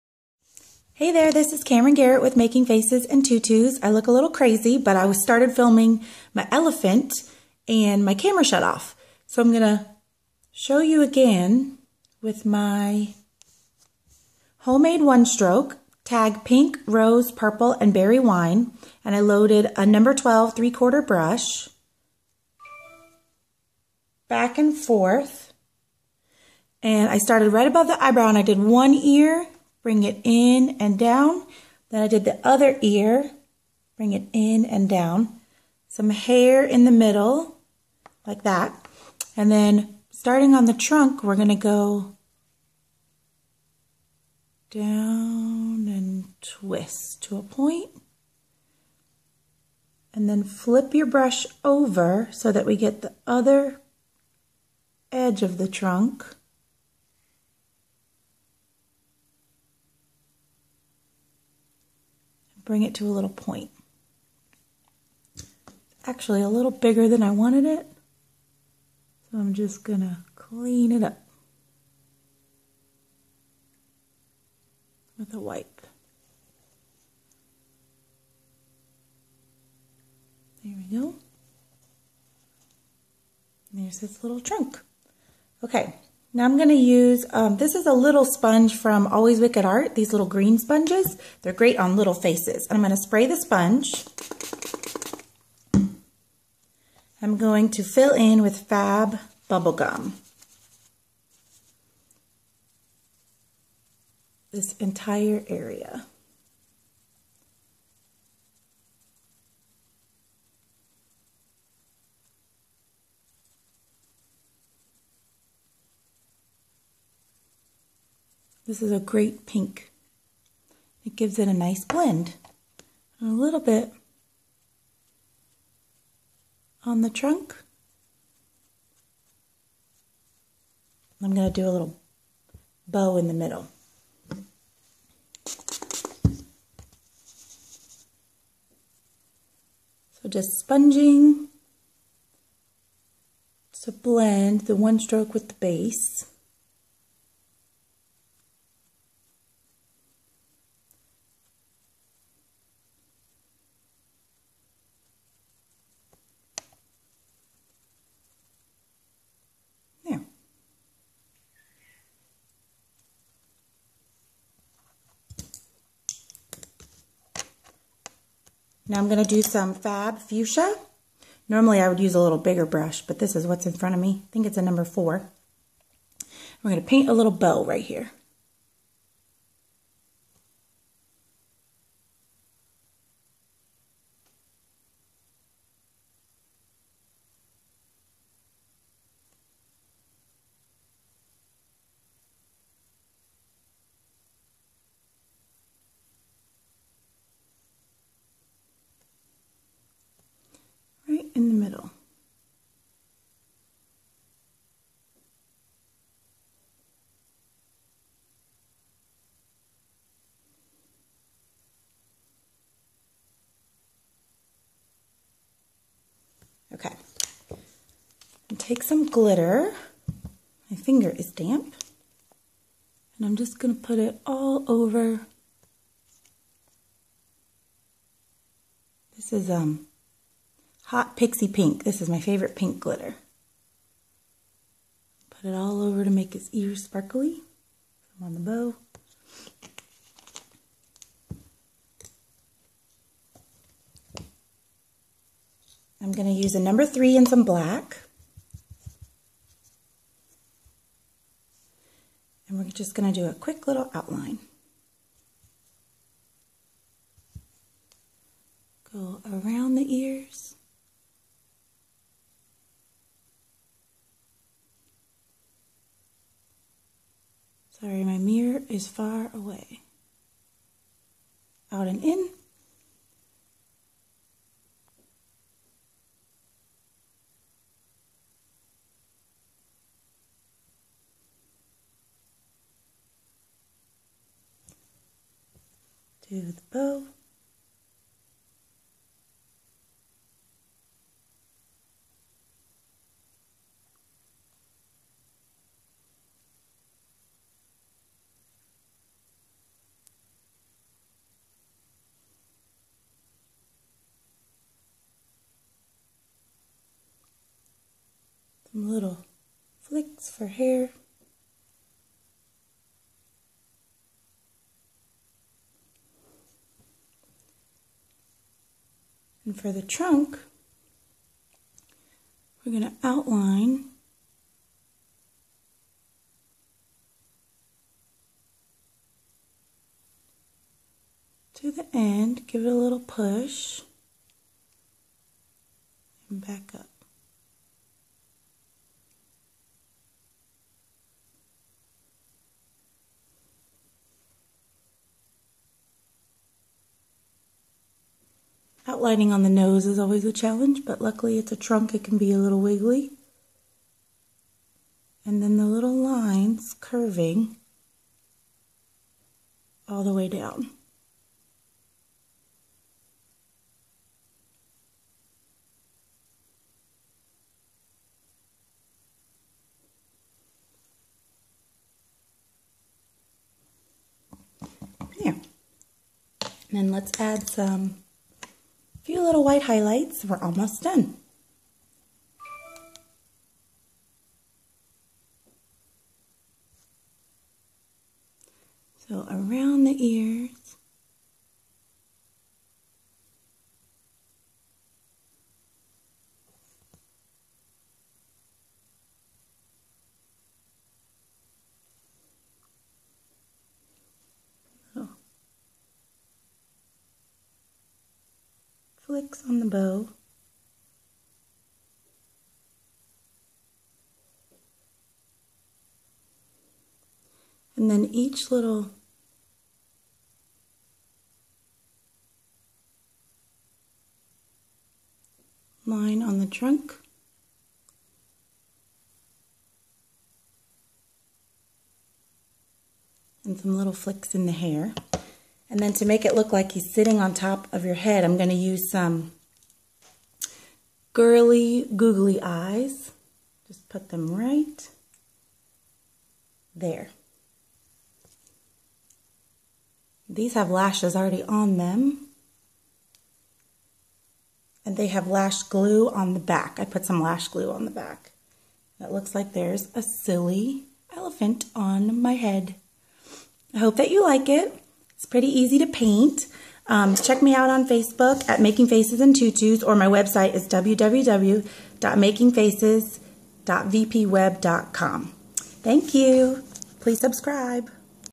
Silly elephant face painting tutorial
Tag homemade one stroke (pink, rose,  purple, berry wine)
Tag 3/4 #12 brush
Fab bubblegum
Black and white
Googly eyes from craft store
Skin safe adhesive